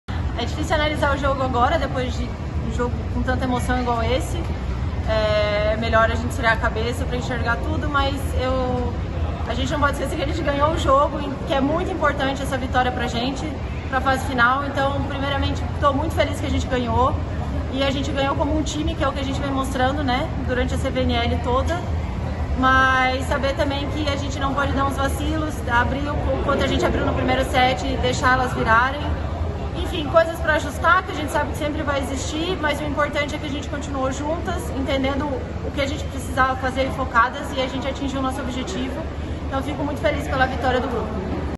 DEPOIMENTO | ROBERTA | LIGA DAS NAÇÕES | 28/06/2023
A levantadora Roberta contribuiu muito com a vitória da seleção feminina de vôlei sobre a Itália por 3 sets a 2, na Tailândia, pela terceira etapa da Liga das Nações 2023. Confira o depoimento da jogadora.